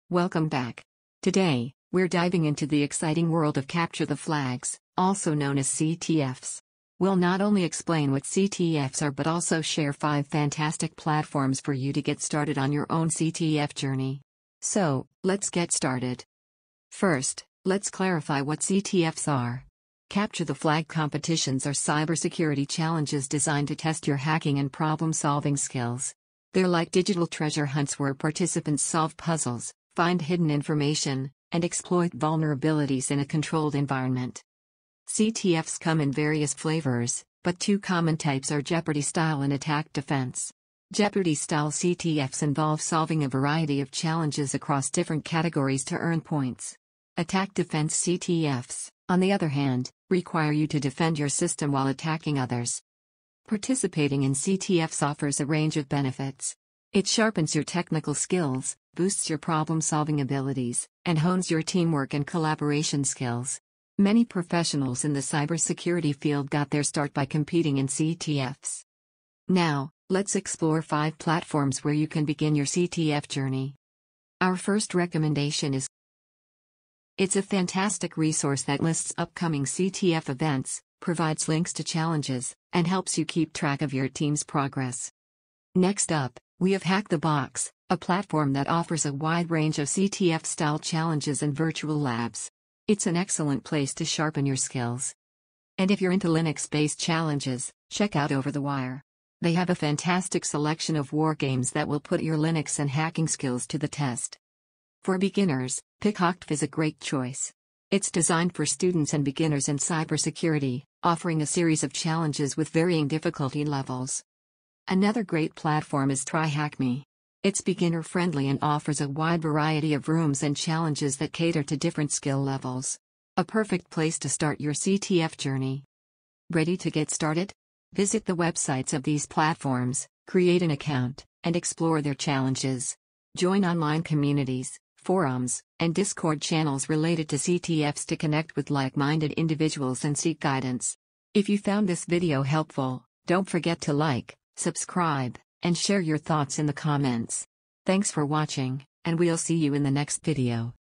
Capture The Flags(CTFs): Hacking Competitions Explained
🔒 Video Description 🔒

Welcome to D@RK! In this video, we're diving deep into the world of Capture The Flags (CTFs). CTFs are thrilling cybersecurity competitions that test and improve your hacking and problem-solving skills. Whether you're a beginner or an experienced pro, CTFs offer a fantastic opportunity to challenge yourself and learn in a controlled environment.

📋 In this video, you can expect:
- An in-depth explanation of what CTFs are and why they matter in the cybersecurity world.
- A breakdown of different types of CTFs, including Jeopardy-style and Attack-Defense.
- Discover the incredible benefits of participating in CTFs, and how they can supercharge your cybersecurity career.
- Most importantly, we'll share the top 5 platforms where you can embark on your CTF journey and start honing your skills.

💻 The 5 Platforms for CTFs we cover include:
1. CTFtime.org - Your CTF event hub.
2. Hack The Box - A virtual playground for CTF enthusiasts.
3. OverTheWire - Perfect for beginners looking to learn the ropes.
4. picoCTF - Tailored for students with approachable challenges.
5. TryHackMe - A platform offering structured learning paths and CTF-style challenges.

Ready to get started? Join us in exploring these fantastic CTF platforms and enhancing your cybersecurity skills. We provide insights, tips, and recommendations to help you kickstart your CTF journey.

💬 Have questions or experiences to share? We'd love to hear from you in the comments section below. Let's foster a vibrant community of cybersecurity enthusiasts.